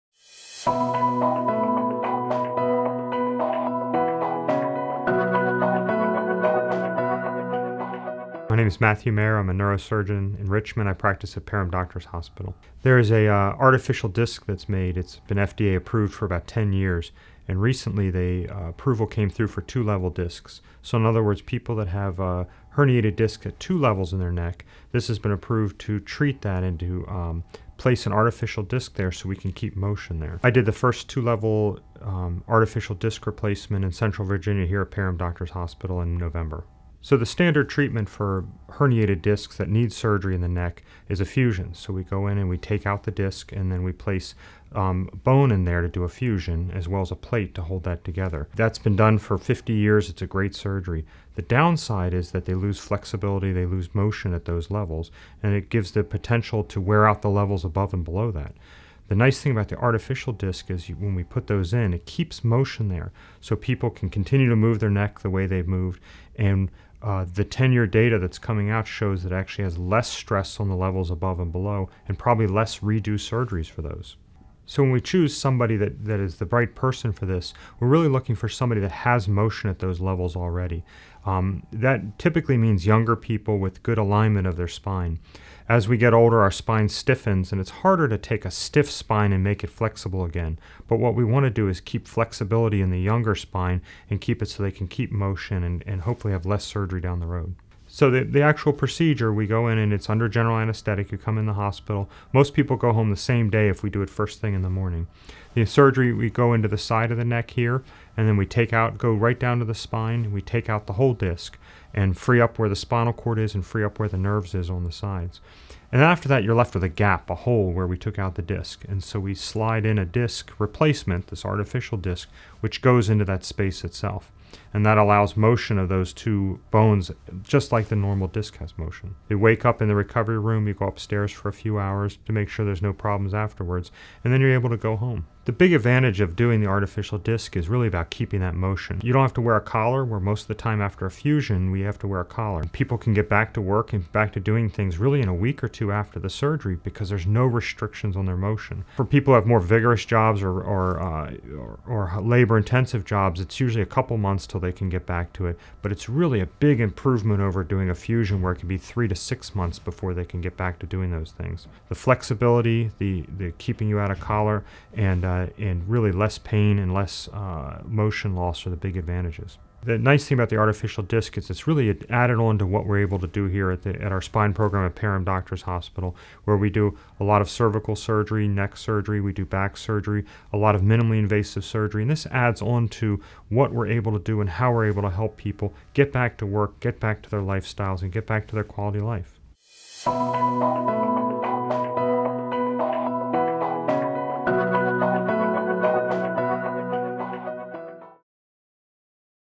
2-Level Artificial Disc Replacement - Parham Doctors' Hospital
Dr. Mayr discusses 2-Level Artificial Disc Replacement.